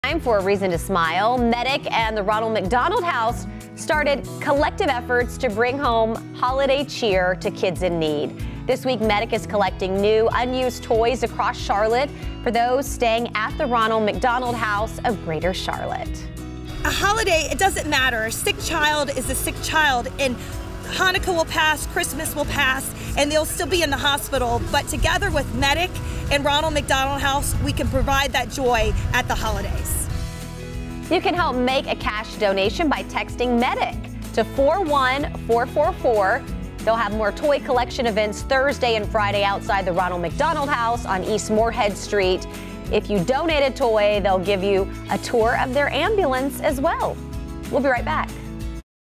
Medic hosts toy drives for Ronald McDonald House
Medic is collecting new, unused toys across Charlotte.